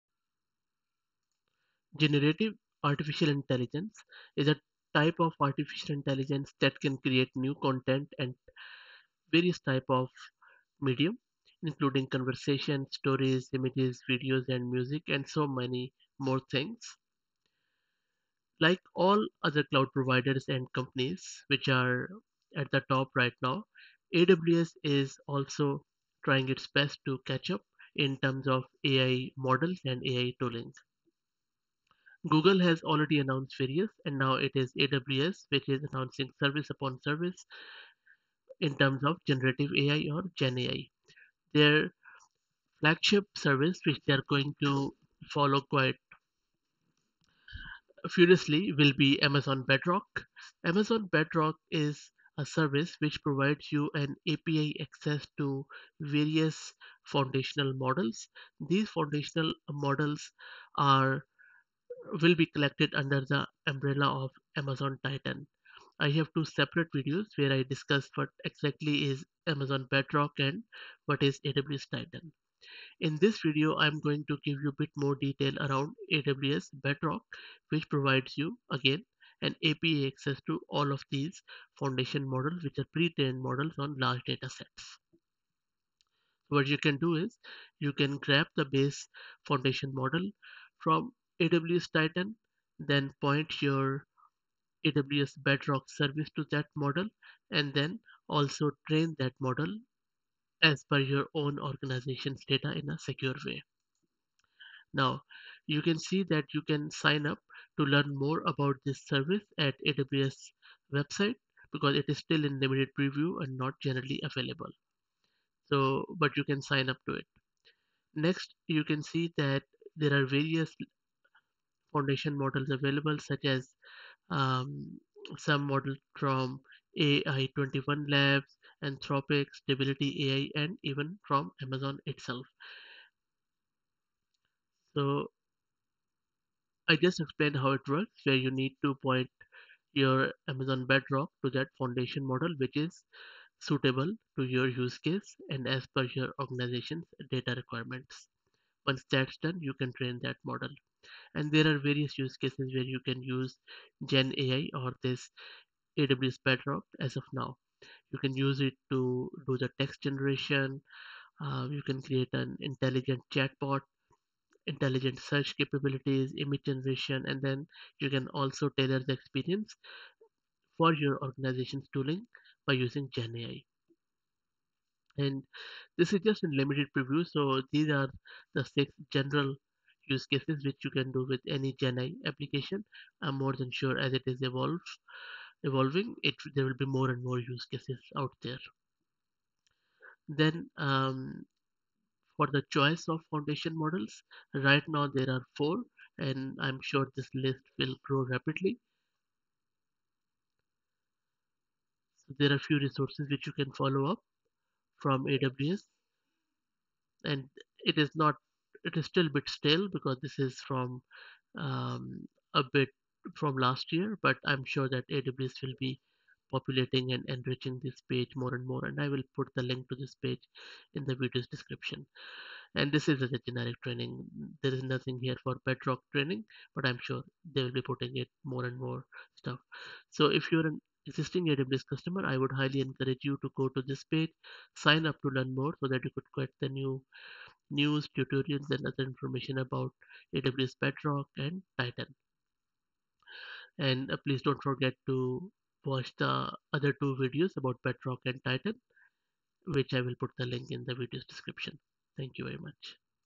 AWS Bedrock for Beginners
This video gives detail about resources for AWS Bedrock 101 in terms of Gen AI for beginners.